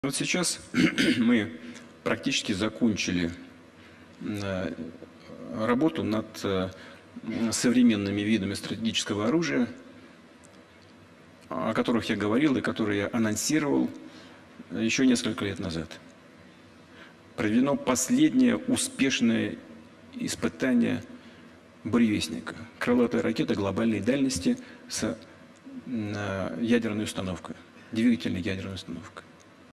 Putin Announces Successful Test of Russian Nuclear-Powered Missile
Russia has successfully tested an experimental nuclear-powered cruise missile, President Vladimir Putin said Thursday. In a speech at a forum of foreign policy experts, Putin announced that Russia has effectively completed the development of the Burevestnik cruise missile and the Sarmat heavy intercontinental ballistic missile, and will work on putting them into production.

“We conducted the last successful test of the Burevestnik nuclear-powered global-range cruise missile,” he said without elaborating. His statement was the first announcement of a successful test of the Burevestnik, which translates as “Storm Petrel.” It was first mentioned by Putin in 2018.

Little is known about the Burevestnik, which was code-named Skyfall by NATO, and many Western experts have been skeptical about it, noting that a nuclear engine could be highly unreliable.